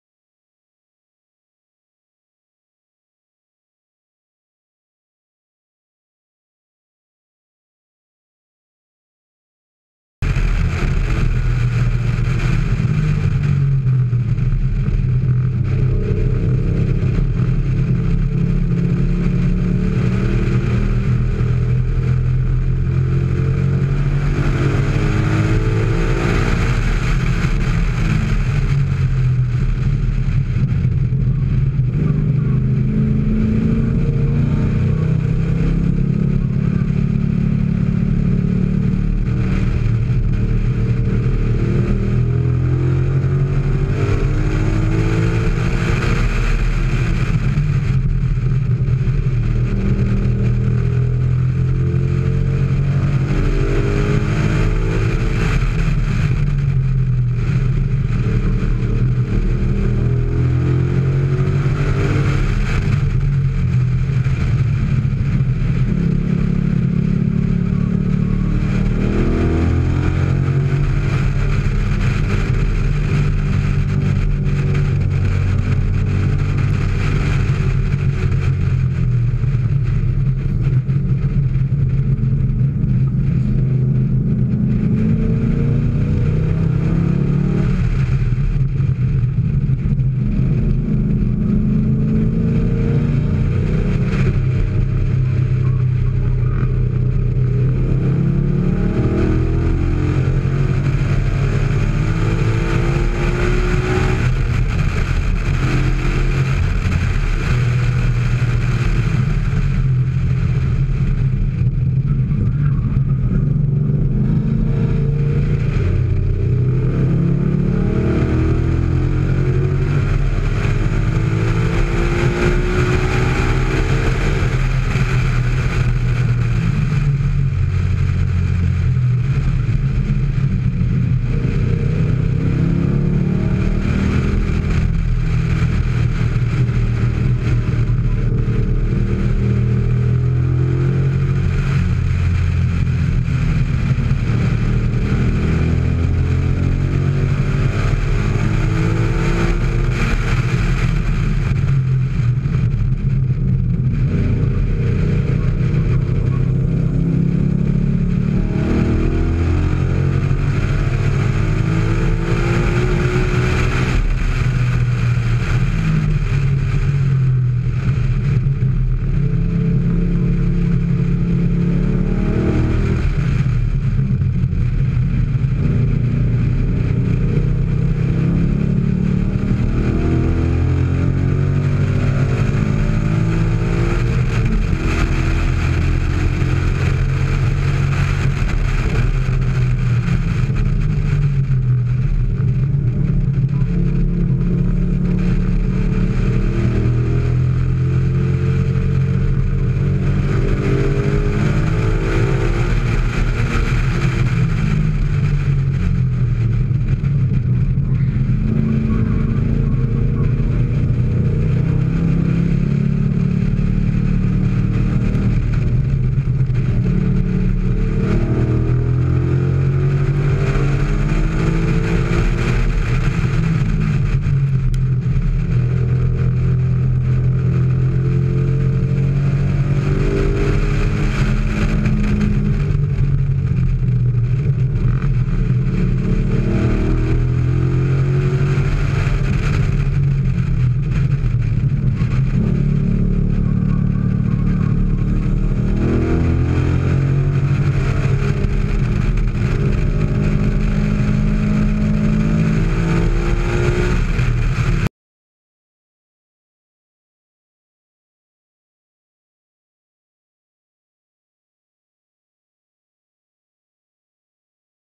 Kenny Brown GT4-RS Mustang Chasing Down a Viper
A short video of Kenny Brown chasing down a Dodge Viper in his GT4-RS Mustang at Autobahn Country Club.